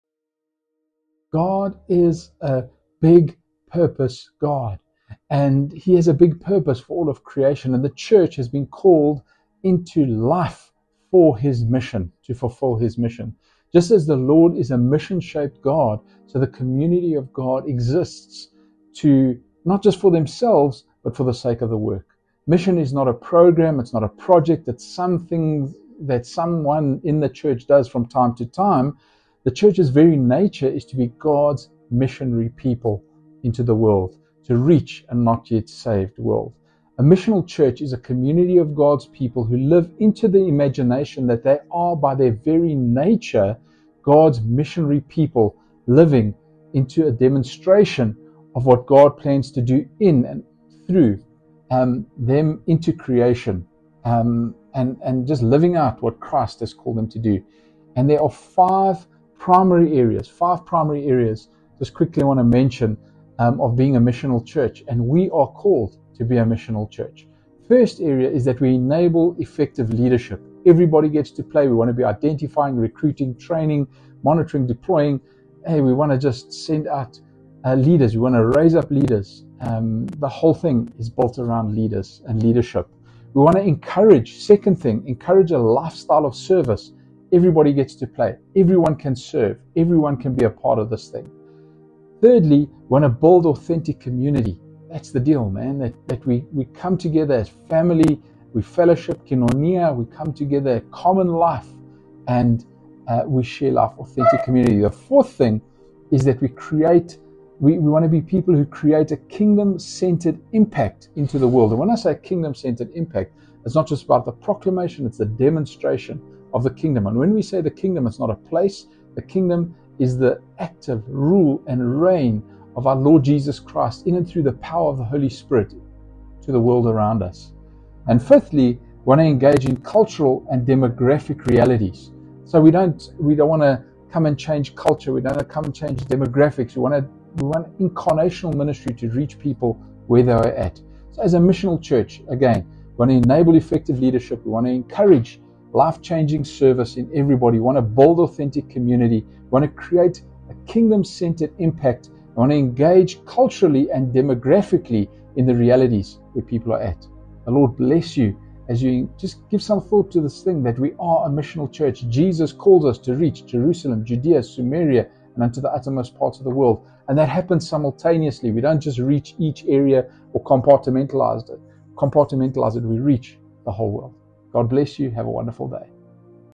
God is about Big Purpose!
The Lord is a mission-shaped God, so the community of God’s people exists, not for themselves but for the sake of the work.